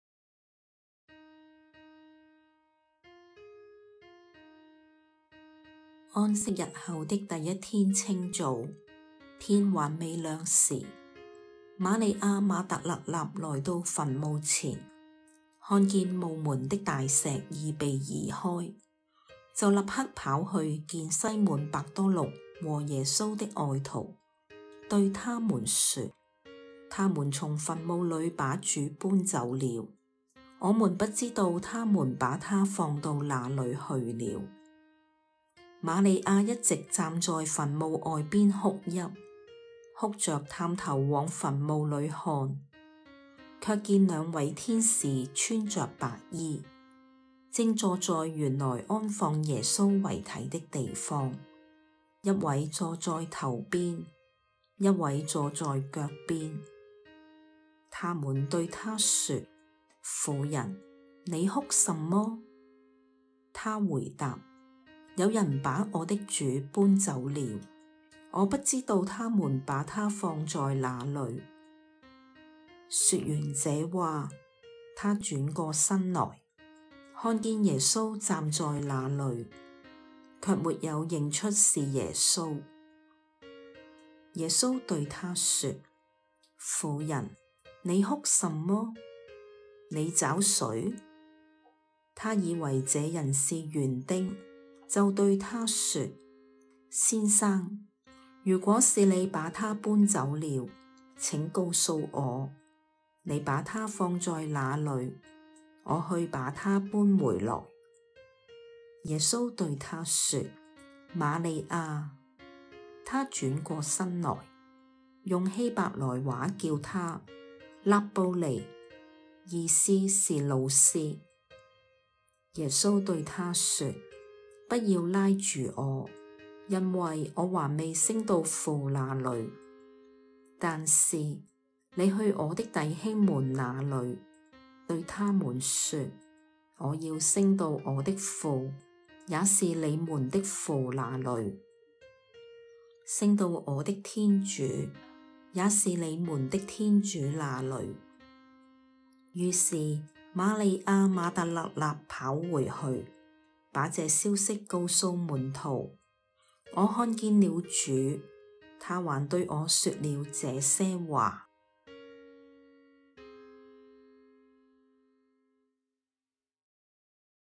若望福音 20:1-2,11-18
若望福音 20:1-2,11-18（女人，你哭什麼？你找誰？）

安息日後的第一天清早，天還未亮時，瑪利亞瑪達肋納來到墳墓前，看見墓門的大石已被移開，就立刻跑去見西滿伯多祿和耶穌的愛徒，對他們說：「他們從墳墓裡把主搬走了，我們不知道他們把祂放到哪裡去了！」
瑪利亞一直站在墳墓外邊哭泣，哭著探頭往墳墓裡看，卻見兩位天使穿著白衣，正坐在原來安放耶穌遺體的地方：一位坐在頭邊，一位坐在腳邊。他們對她說：「婦人！你哭什麼？」她回答：「有人把我的主搬走了，我不知道他們把祂放在那裡！」說完這話，她轉過身來，看見耶穌站在那裡，卻沒有認出是耶穌。耶穌對她說：「婦人，你哭什麼？你找誰？」她以為這人是園丁，就對祂說：「先生，如果是你把祂搬走了，請告訴我，你把祂放在哪裡，我去把祂搬回來。」耶穌對她說：「瑪利亞！」她轉過身來，用希伯來話叫祂：「辣步尼！」意思是「老師」。耶穌對她說：「不要拉住我！因為我還未升到父那裡。但是，你去我的弟兄們那裡，對他們說：『我要升到我的父，也是你們的父那裡；升到我的天主，也是你們的天主那裡。』」於是瑪利亞瑪達肋納跑回去，把這消息告訴門徒：「我看見了主！祂還對我說了這些話。」